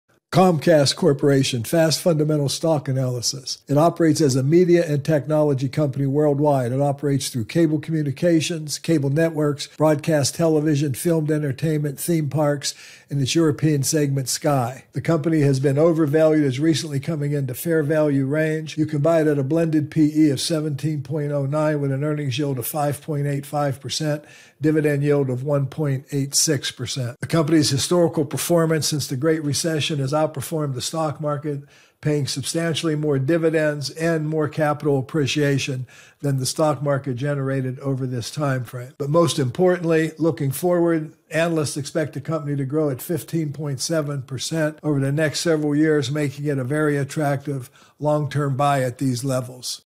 Stock Analysis Comcast Corp (CMCSA)
FAST Fundamental Stock Analysis on Comcast Corp (CMCSA)
$CMCSA

Comcast Corp. is a media, entertainment, and communications company, which engages in the provision of video, Internet, and phone services. It operates through the following segments: Cable Communications, Cable Networks, Broadcast Television, Filmed Entertainment, Theme Parks and Sky.